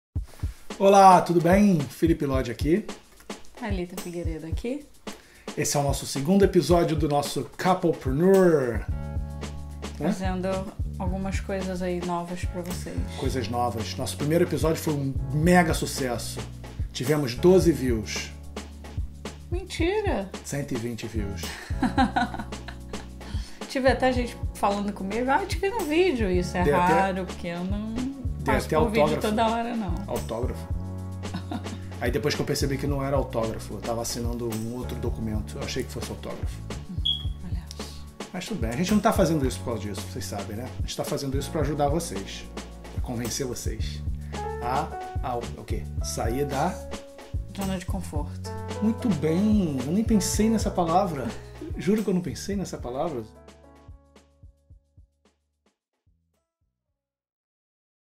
(In Portuguese) Os Fundadores da WorkFlow ICT Discutem Educação Para Imigrantes na Irlanda. (Teaser)
(In Portuguese) Thalita Figueiredo and Felipe Lodi, fundadores da WorkFlow ICT, falam sobre a educação na Irlanda, as categorias de ensino, as atividades extra-curriculares e quanto custa isso tudo. Assista o vídeo completo no CouplePreneurs in Europe da WorkFlow ICT